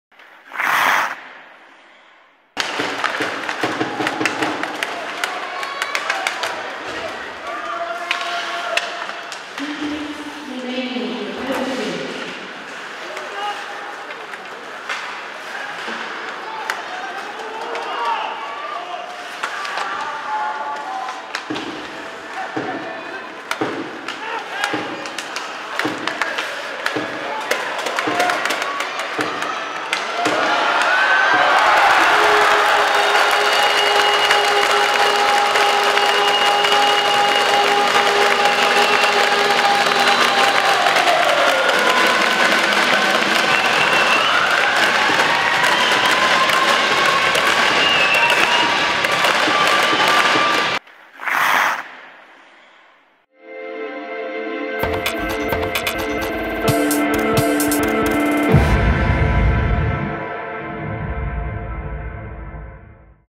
GOAL | HIKOSAKA Yu | 2017. 1. 29 | Nikko Icebucks vs Daemyung Killerwhales
GOAL
HIKOSAKA Yu
Nikko Icebucks vs Daemyung Killerwhales
Asia League Ice Hockey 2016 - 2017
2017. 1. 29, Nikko